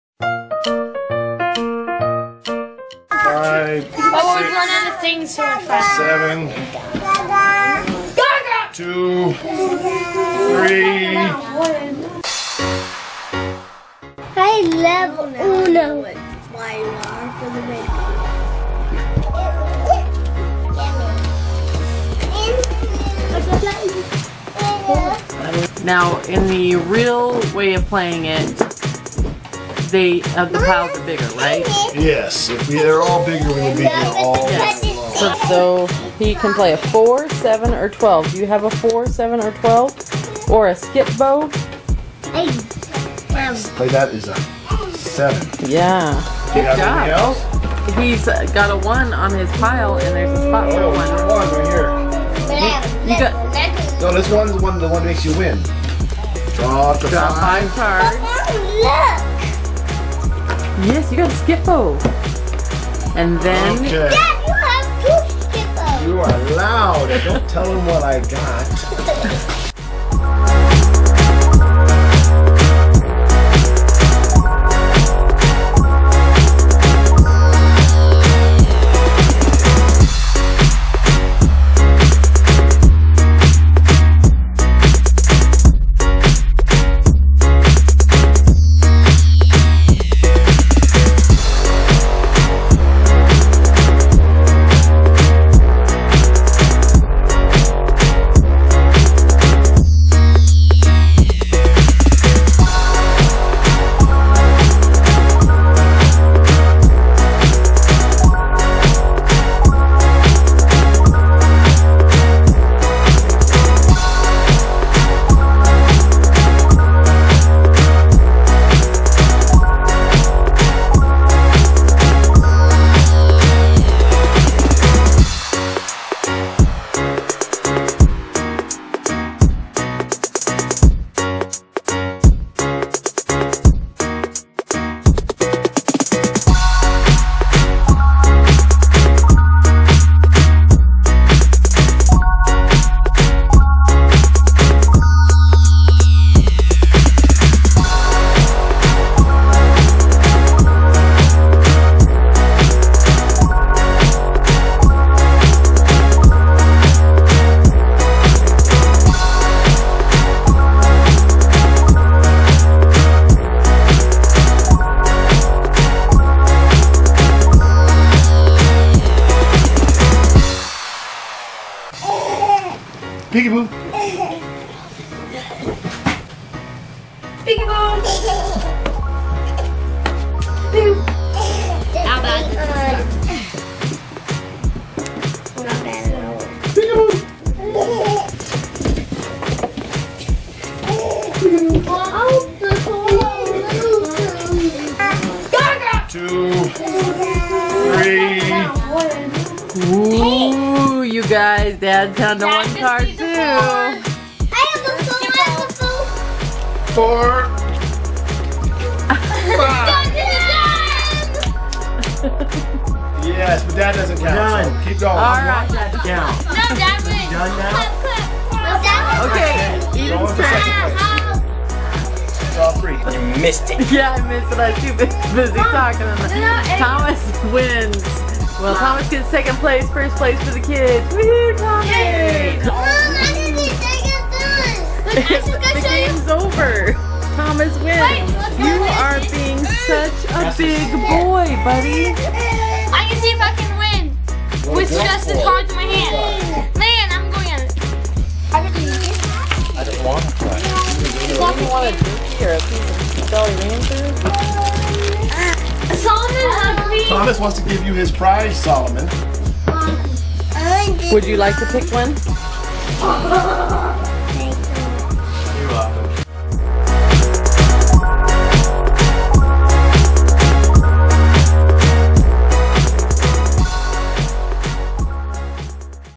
Skip-Bo║ Toddlers to Teens │ Large Family Game Night
Tuesday night is Game Night at our house and tonight we're playing SkipBo (Grandma Kallemeyn's favorite!) with some modifications to fit our family of 10. It can be noisy, crazy, boring and fun!

Card Holders

Mills Family
Moorcroft, Wyoming 82721